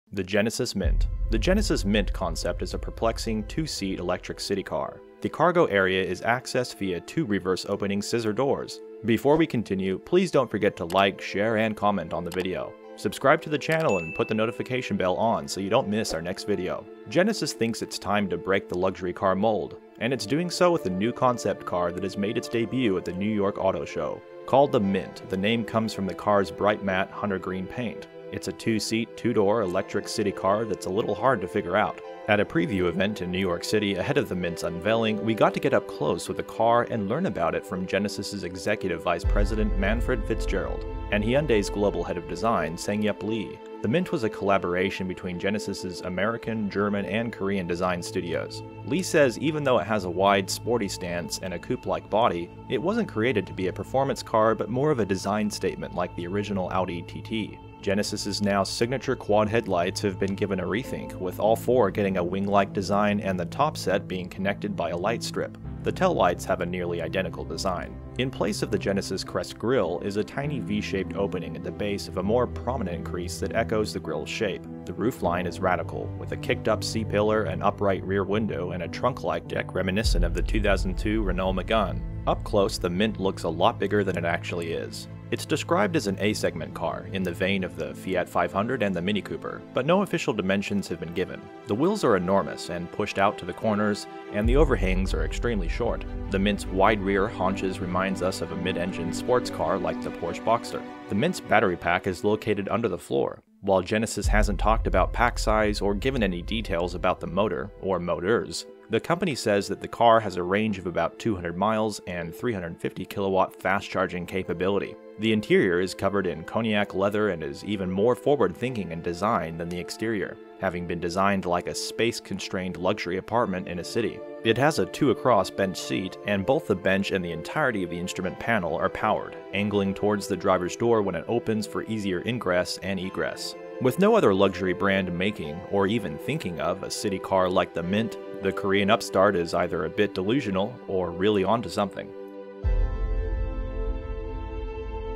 The New Genesis Mint Concept Supercar
Genesis is challenging the conventional design of luxury cars with a new concept vehicle showcased at the New York auto show. Unveiled under the name "Mint" due to its distinctive matte Hunter Green paint, this electric city car features a unique two-seat, two-door configuration, creating a somewhat puzzling yet intriguing design. During a preview event in New York City preceding the Mint's official debut, we had the opportunity to closely examine the vehicle. Additional insights were provided by Genesis's executive vice president, Manfred Fitzgerald, and Hyundai's global head of design, SangYup Lee. It's important to note that the Mint is the result of a collaborative effort involving Genesis's design studios in the United States, Germany, and Korea.